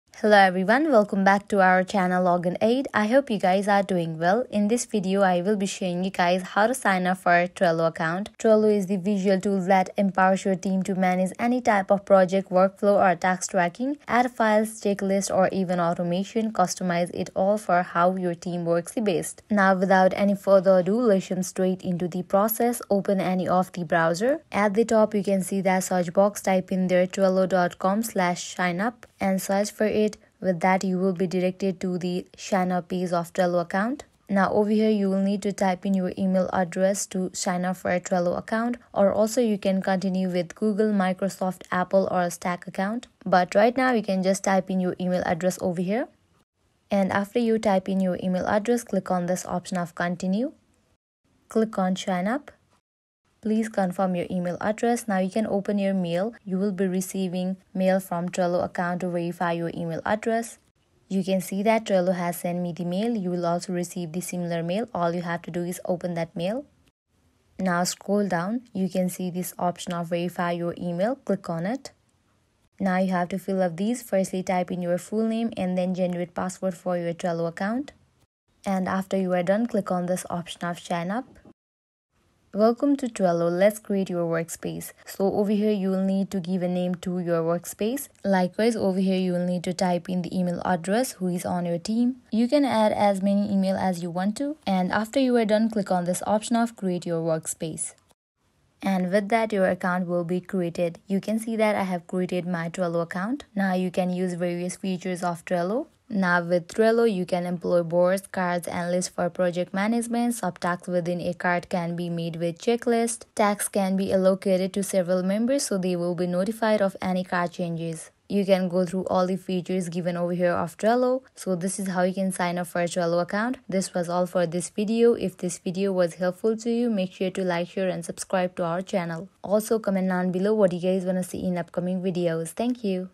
Trello Account Registration, Sign Up Guide 2023 | Create Trello Account
A video guide for the process to create Trello account.